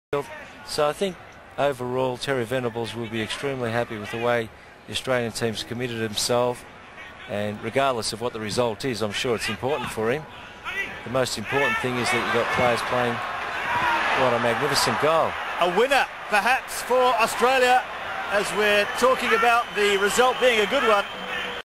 Kevin Muscat scores against Hungary 1997
Kevin Muscat scores an unlikely winner against Hungary, in Budapest 1997